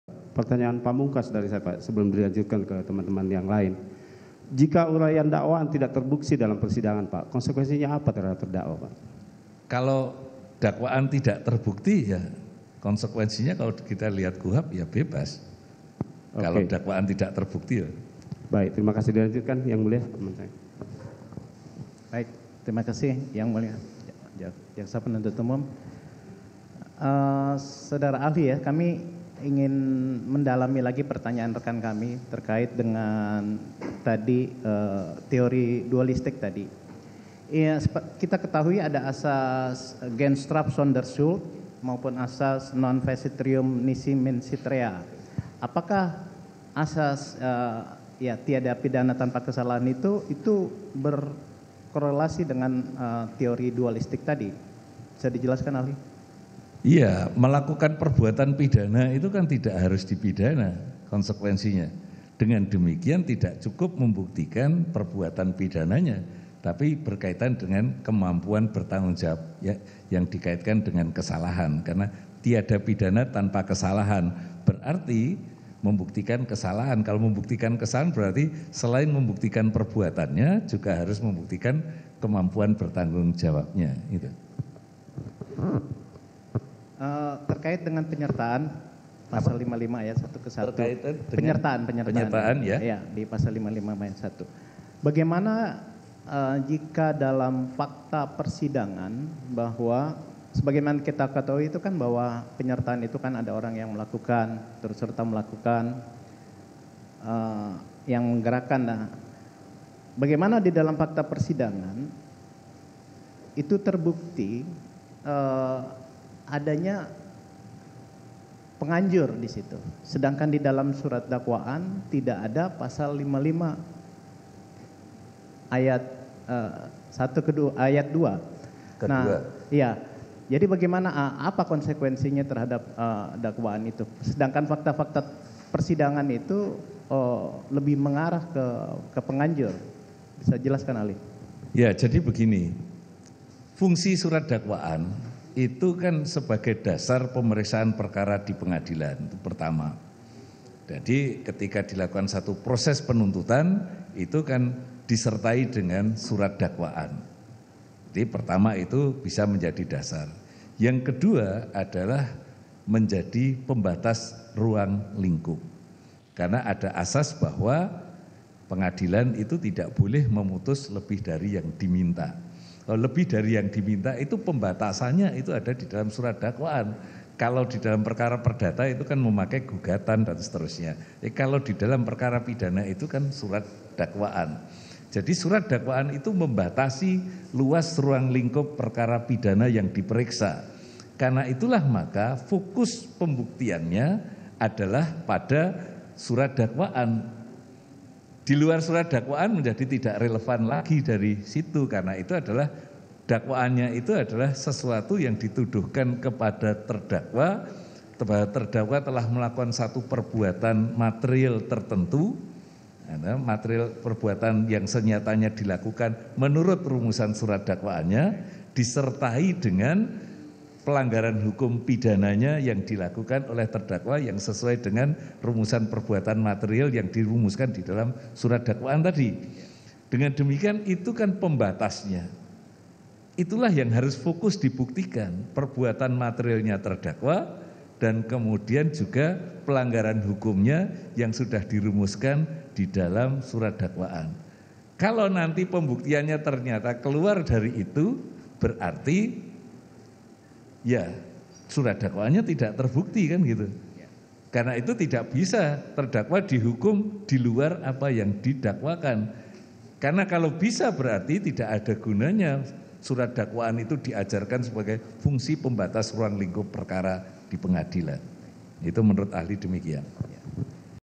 Ditanyai soal Konsekuensi Dakwaan Jika Tidak Terbukti, Ini Jawaban Saksi Ahli Hukum Pidana!
JAKARTA, KOMPAS.TV - Ahli Hukum Pidana, Muhammad Arif Setiawan menjelaskan soal surat dakwaan.

Ahli menyebut bahwa fungsi surat dakwaan sebagai dasar pemeriksaan perkara di pengadilan.

Ahli juga mengatakan jika pembuktian keluar dari isi surat dakwaan, terdakwa tidak bisa dihukum diluar apa yang didakwakan.

''Kalau dakwaan tidak terbukti, konsekuensinya ya bebas,'' ujar Arif.

Kesaksian ini diberikan dalam sidang lanjutan kasus Sambo dengan terdakwa Kuat Ma'ruf dan Ricky Rizal di PN Jaksel, Senin (2/01).